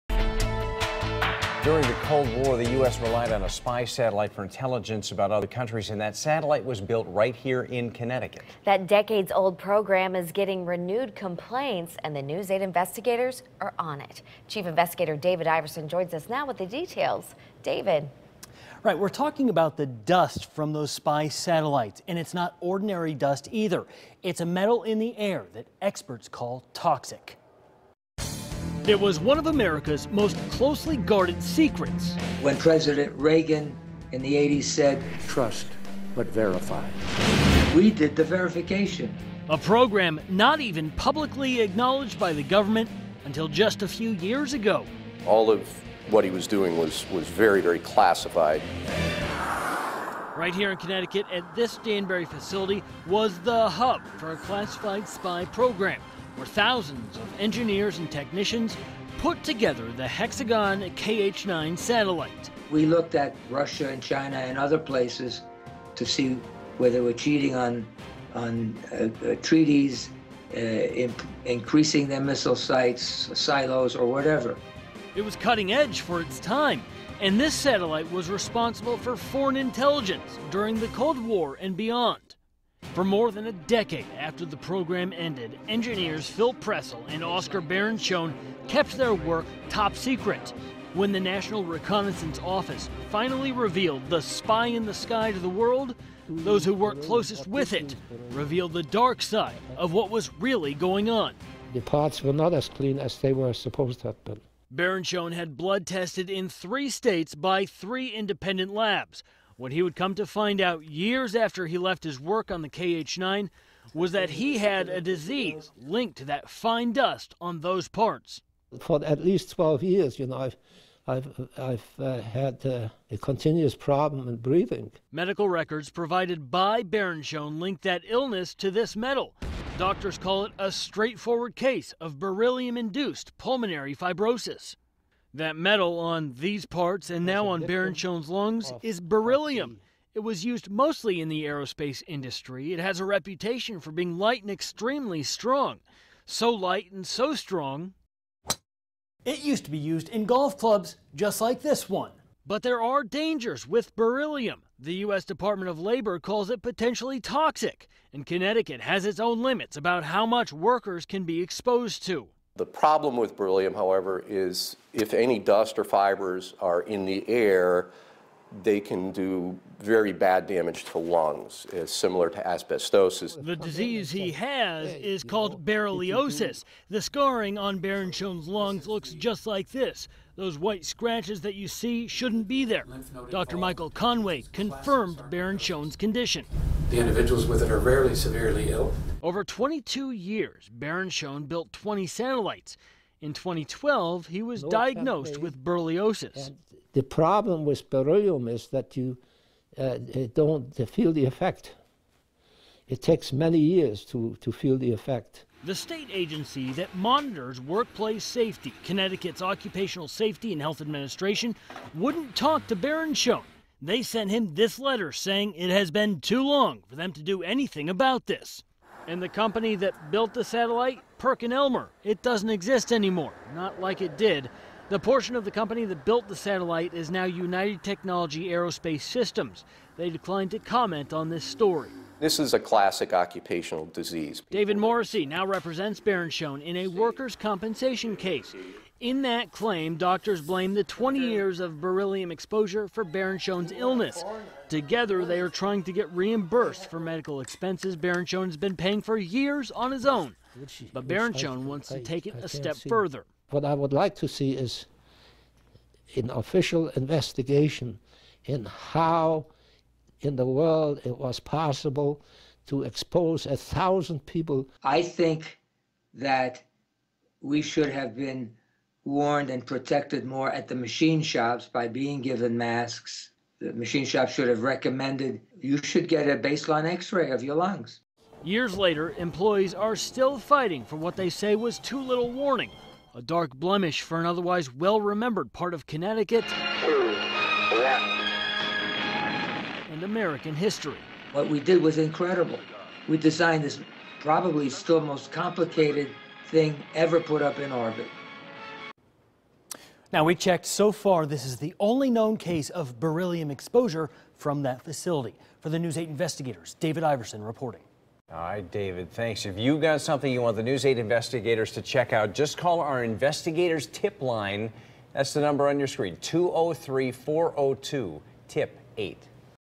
CT Classified Spy Satellite blamed for chemical exposure
During the Cold War, the United States relied on a spy satellite for intelligence about other countries.